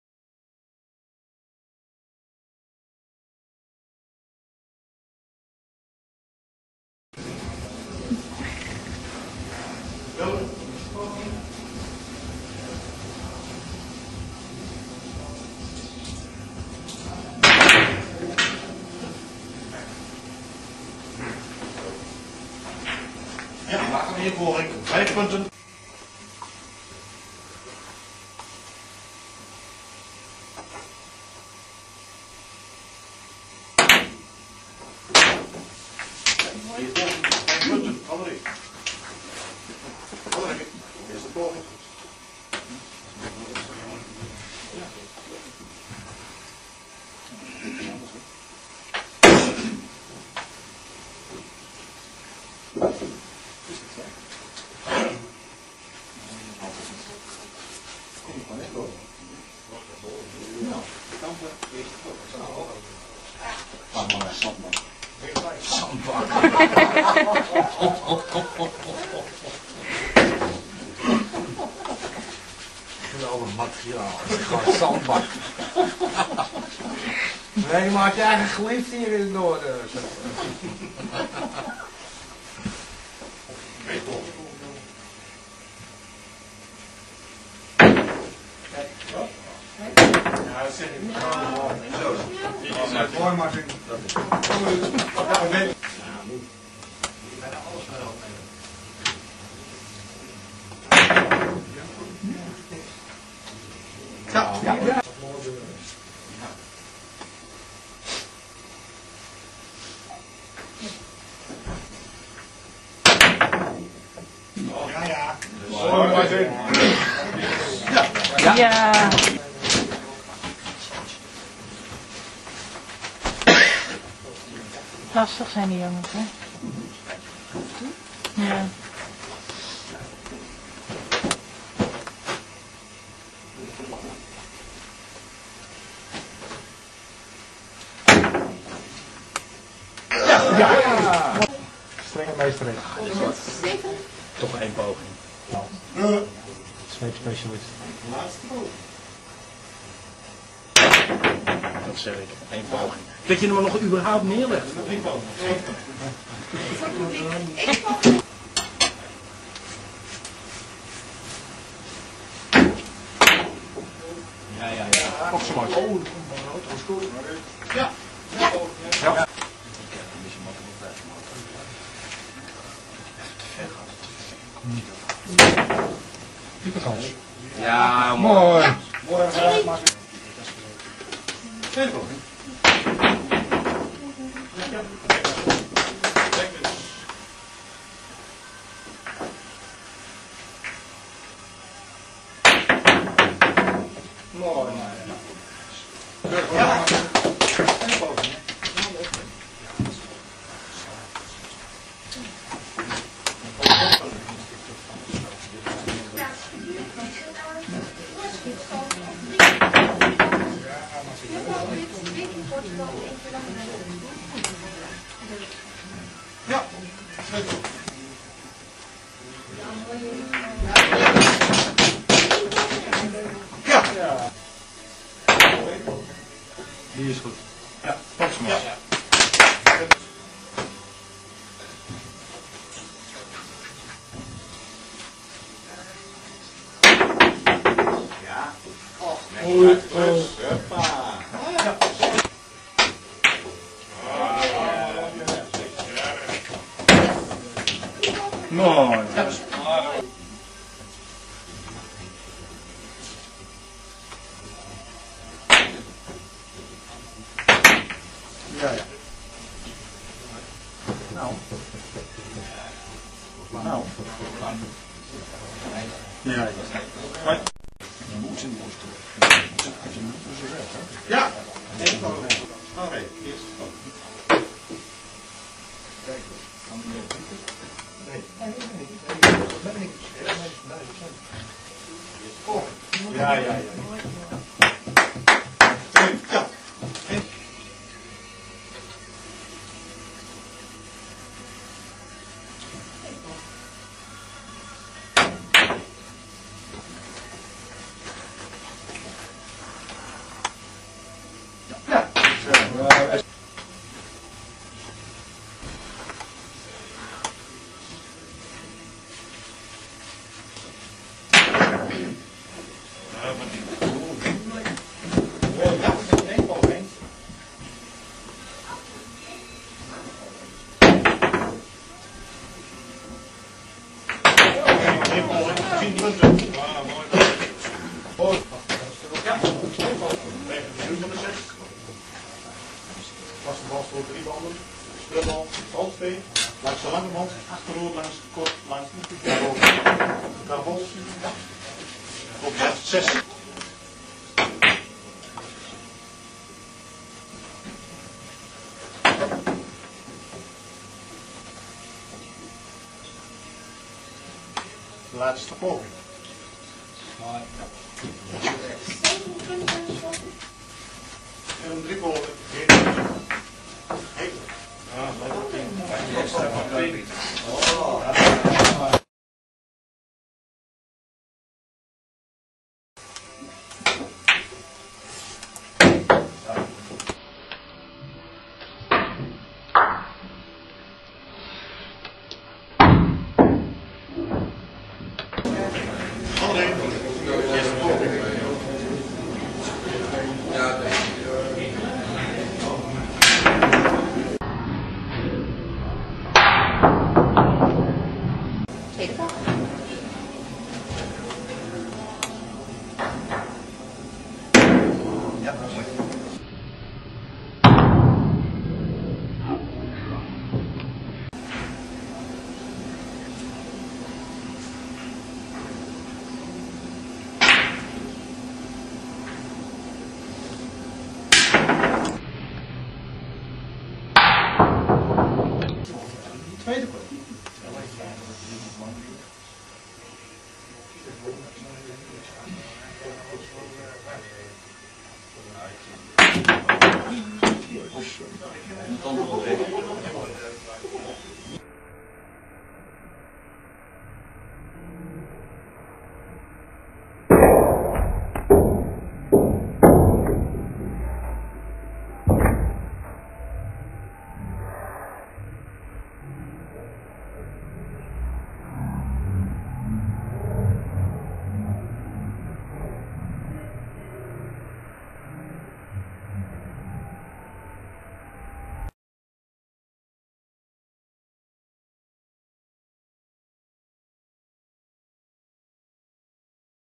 Trickshot billiards Grand Prix 1
Biljart artistiek is een spectaculaire spelsoort binnen het biljarten. Er komt veel techniek en kennis bij kijken om de speelbal je wil op te leggen.

Artistic billiards or artistik bilardo or billar artistico is a spectacular game of billiards.
A short clip about the first Grand Prix Biljart Artistiek played by Martin van Rhee and others in Midwolda,Nederland.
Like the second Grand Prix this was an official tournament.